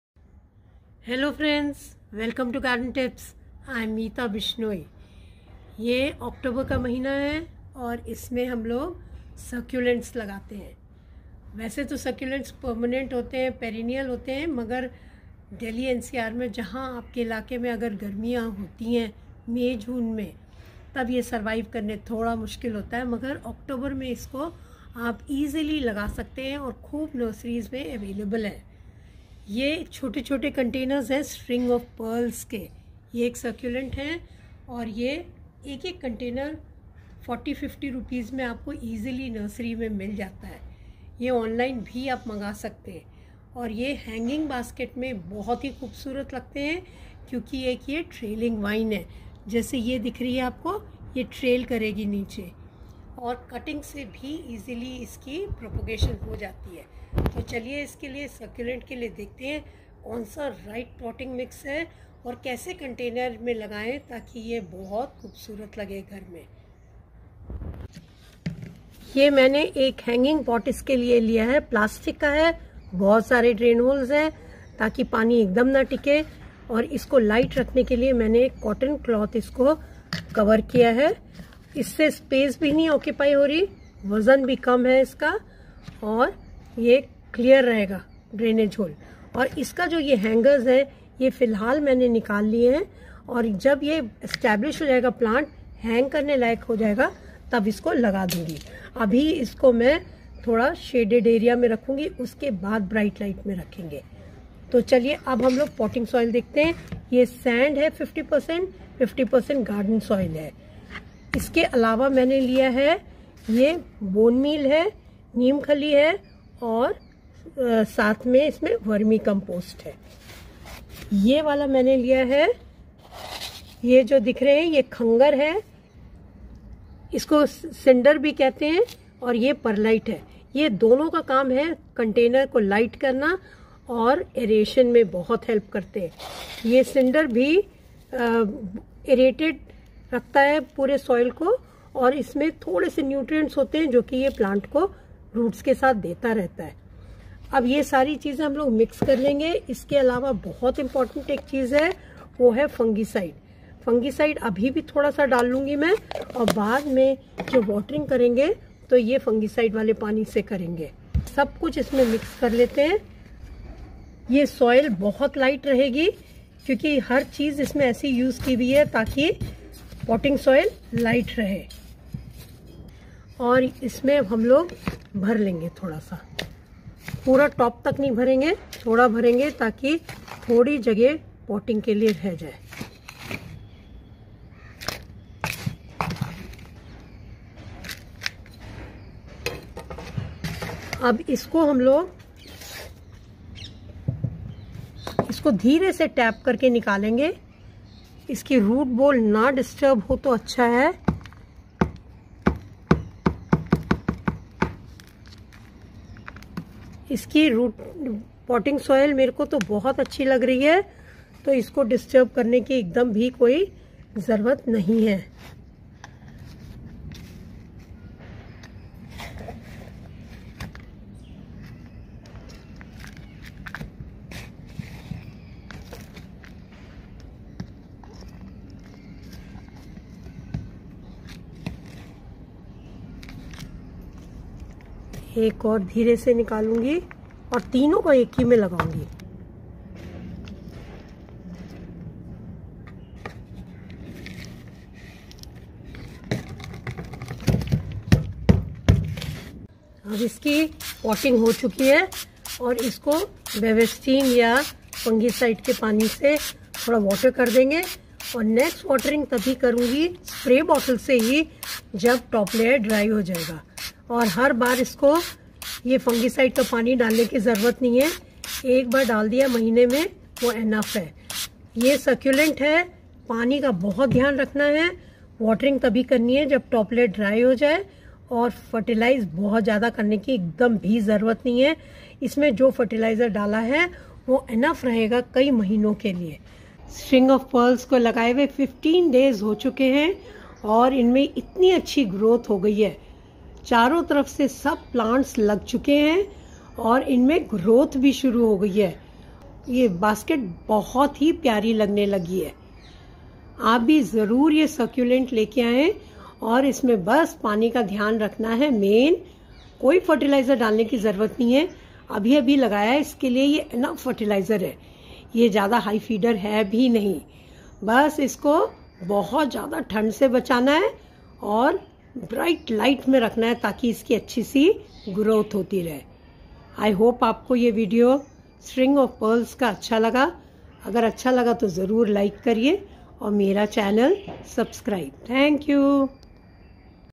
Make hanging basket of String of Pearls with me
Succulent Atring of Pearls repotting in Hanging Basket. Potting soil details & care